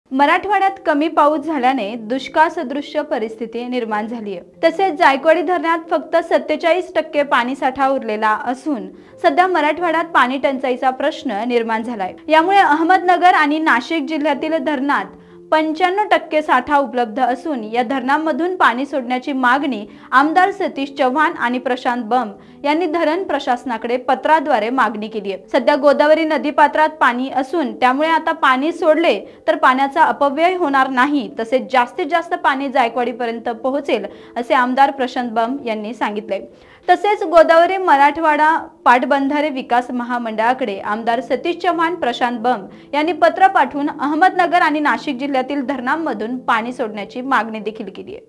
Maharashtra News: आमदार सतीश चव्हाण यांचे गोदावरी मराठवाडा पाटबंधारे विकास महामंडळाला पत्र#dam#news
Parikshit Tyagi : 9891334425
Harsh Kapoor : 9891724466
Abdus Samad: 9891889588